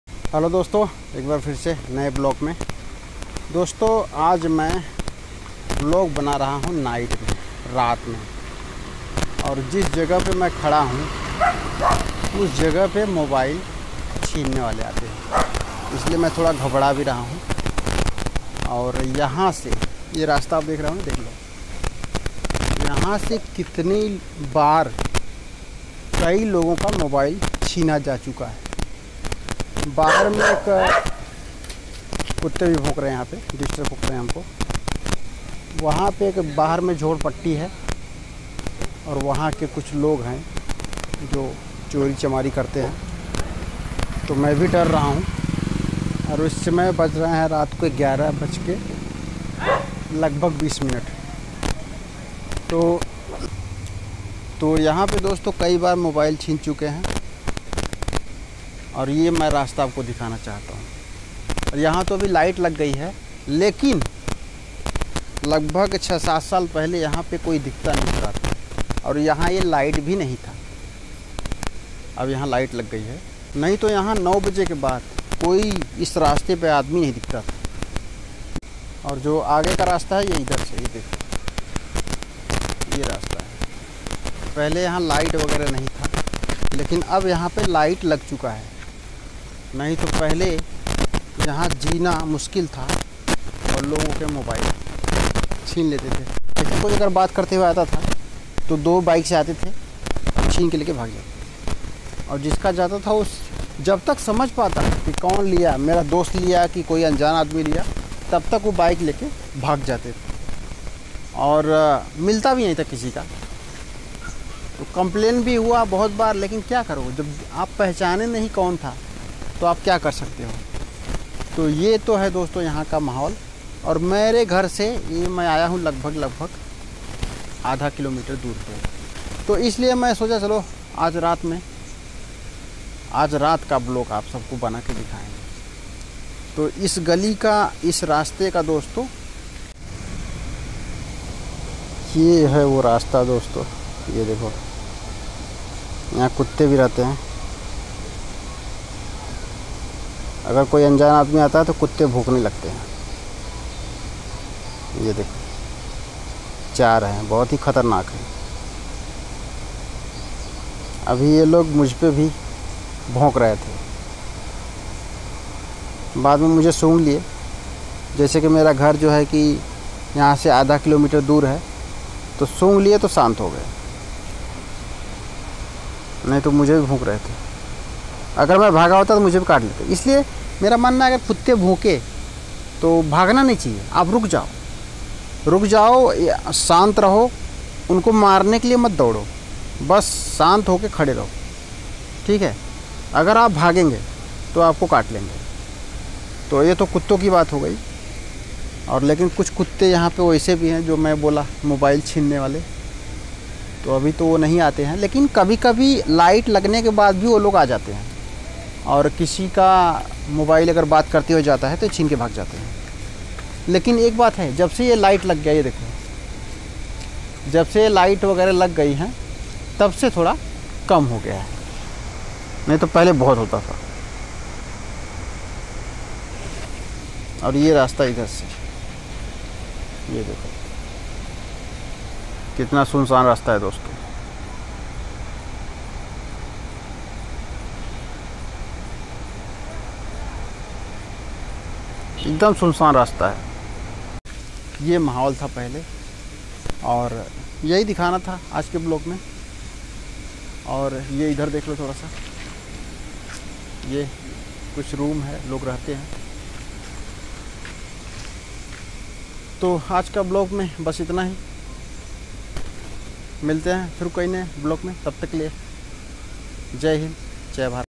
इस रास्ते पर छीनते हैं मोबाइल चोर ब्लॉग वीडियो || Mobile Thieves Snatch Blog Videos On This Path.
इस रास्ते पर चोरी करते हैं मोबाइल चोर

Mobile thieves snatch blog videos on this path ||

chori Karta hua chor mobile blog video